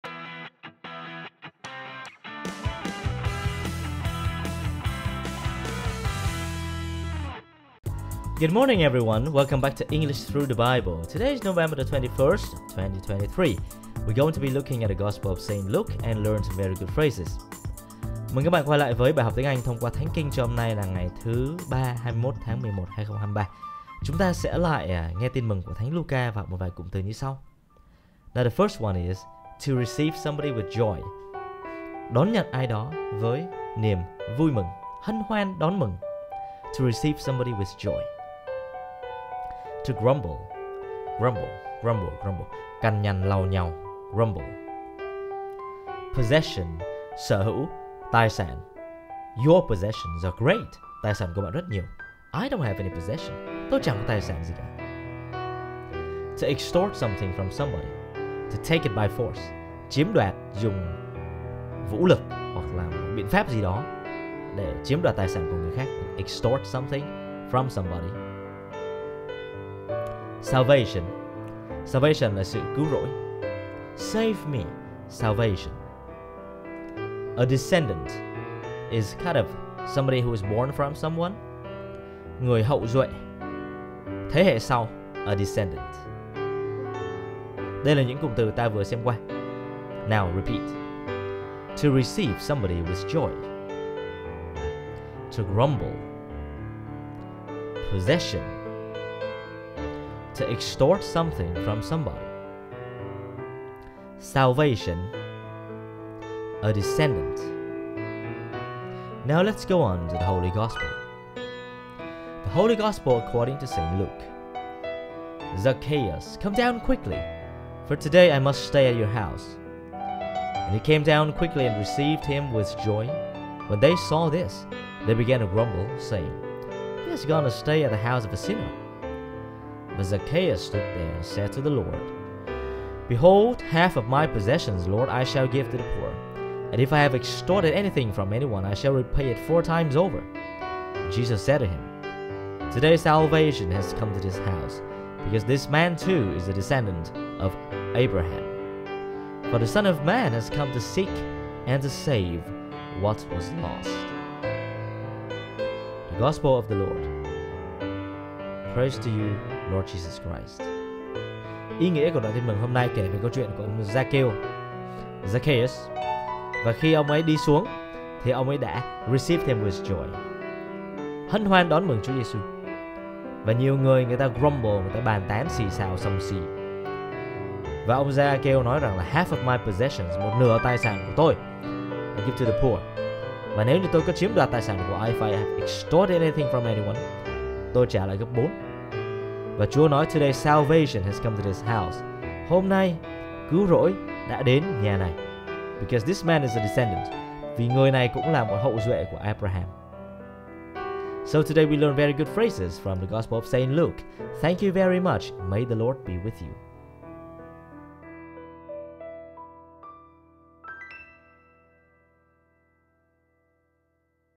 English through the Bible 21 11 2023
To receive sb with joy (v)
To grumble (v)
Possession (n)
To extort sth from sb (v)
Salvation (n)
A descendant (n)
Gospel
Lk 19:1-10
At that time Jesus came to Jericho and intended to pass through the town.
Now a man there named Zacchaeus,
who was a chief tax collector and also a wealthy man,
was seeking to see who Jesus was;
but he could not see him because of the crowd,
for he was short in stature.
So he ran ahead and climbed a sycamore tree in order to see Jesus,
who was about to pass that way.
When he reached the place, Jesus looked up and said,
"Zacchaeus, come down quickly,
for today I must stay at your house."
And he came down quickly and received him with joy.
When they saw this, they began to grumble, saying,
"He has gone to stay at the house of a sinner."
But Zacchaeus stood there and said to the Lord,
"Behold, half of my possessions, Lord, I shall give to the poor,
and if I have extorted anything from anyone
I shall repay it four times over."
And Jesus said to him,
"Today salvation has come to this house
because this man too is a descendant of Abraham.
For the Son of Man has come to seek
and to save what was lost."
Tin Mừng Chúa Giêsu Kitô theo Thánh Luca.
Khi ấy, Chúa Giêsu vào Giêricô và đi ngang qua thành, thì kìa, có một người tên là Giakêu, ông thủ lãnh những người thu thuế và là người giàu có. Ông tìm cách để nhìn xem Chúa Giêsu là người thế nào, nhưng không thể được, vì người ta đông quá, mà ông lại thấp bé. Vậy ông chạy lên trước, trèo lên một cây sung để nhìn xem Người, vì Người sắp đi ngang qua đó. Khi vừa đến nơi, Chúa Giêsu nhìn lên và trông thấy ông ấy, nên Người bảo ông rằng: "Hỡi Giakêu, hãy xuống mau, vì hôm nay Ta phải lưu lại tại nhà ngươi". Ông vội vàng trụt xuống và vui vẻ đón tiếp Người. Mọi người thấy vậy, liền lẩm bẩm rằng: "Ông này lại đến trọ nhà một người tội lỗi". Ông Giakêu đứng lên thưa cùng Chúa rằng: "Lạy Ngài, tôi xin bố thí nửa phần của cải tôi cho kẻ khó, và nếu tôi có làm thiệt hại cho ai điều gì, tôi xin đền gấp bốn". Chúa Giêsu bảo ông ấy rằng: "Hôm nay nhà này được ơn cứu độ, bởi người này cũng là con cái Abraham. Vì chưng, Con Người đến tìm kiếm và cứu chữa điễu gì đã hư mất".
Đó là lời Chúa.